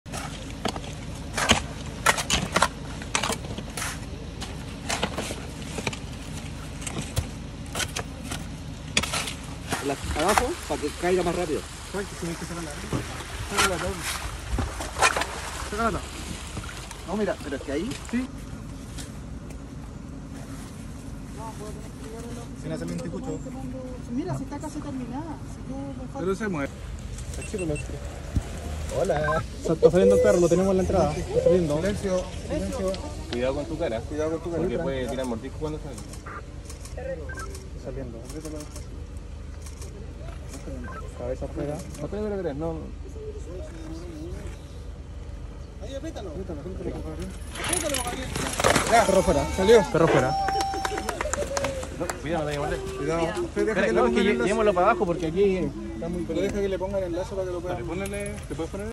Bomberos rescatan a perrita atrapada en tubo recolector de agua en Quilpué
Voluntarios de la Primera Compañía de Bomberos Quilpué, junto a funcionarios del municipio, rescataron a una perrita que había quedado atrapada en un tubo recolector de agua. Tras casi 3 horas de trabajo, los Bomberos lograron de forma exitosa el rescate, y funcionarios municipales la trasladan a la veterinaria municipal.